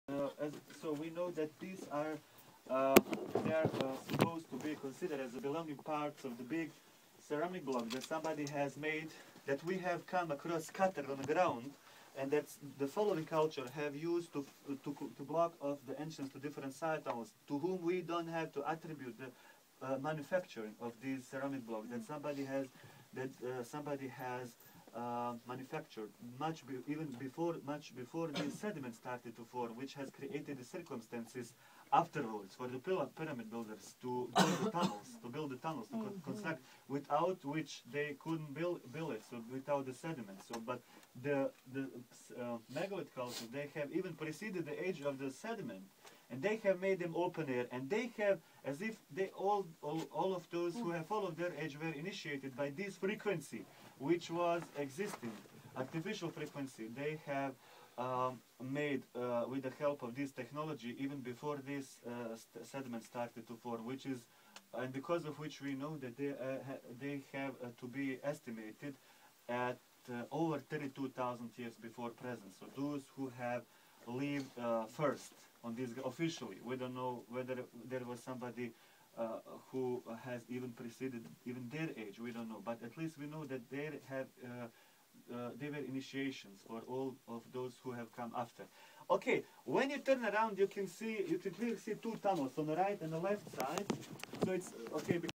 Ravne Tunnels & Megaliths
Short history Lesson on the manufacturers of the Ceramic encased Quartz Megaliths.